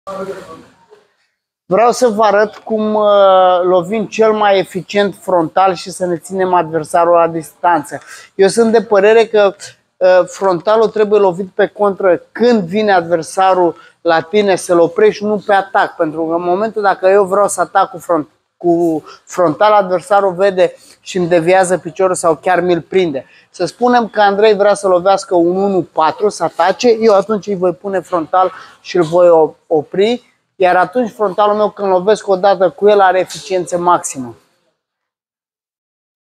Pitbull Atodiresei te invata cum sa lovesti cel mai eficient frontala de picior
Pitbull Atodiresei te invata cum sa lovesti cel mai eficient frontala de picior - Tehnici

🥋 Vino la Absoluto si primesti CADOU primul antrenament GRATUIT!
📌 Te asteptam la antrenamente la Absoluto Unirii, Cotroceni si Veranda Mall - nivelul 2!